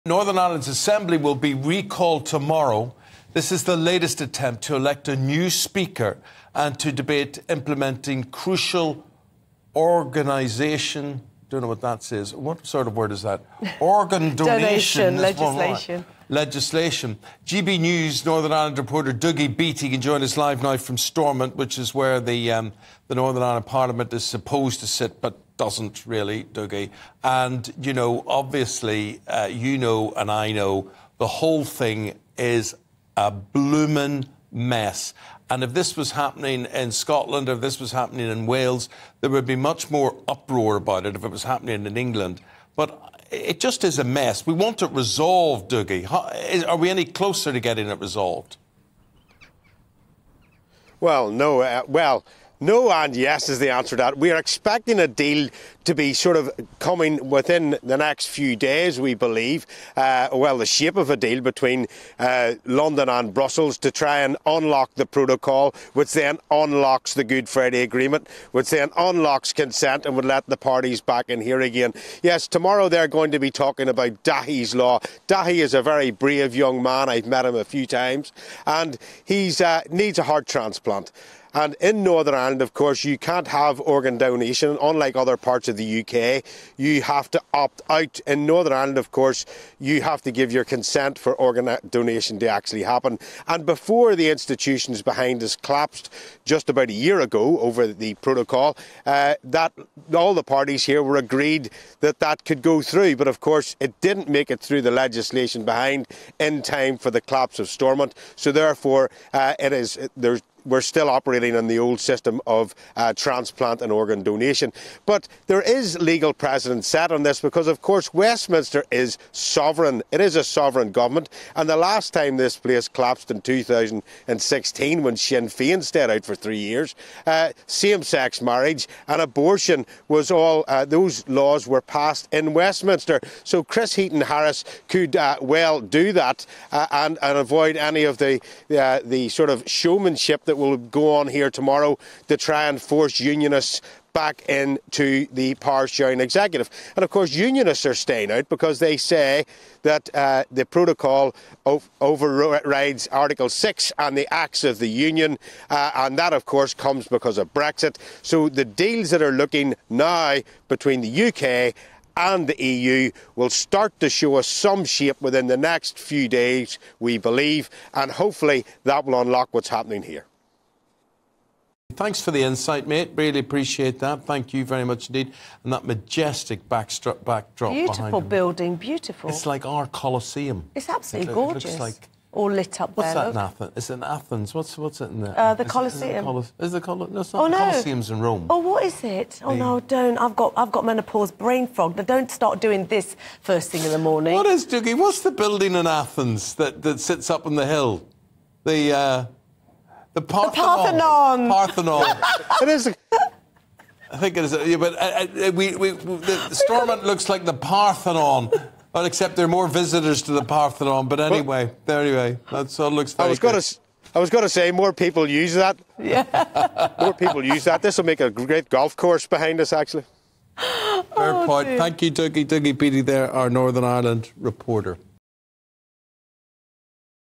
Northern Ireland Assembly could meet tomorrow to pass crucial opt-out organ donation laws
Dougie Beattie reports as the Northern Ireland assembly could recall tomorrow, in an attempt to elect a new speaker and to debate implementing crucial organ-donation legislation.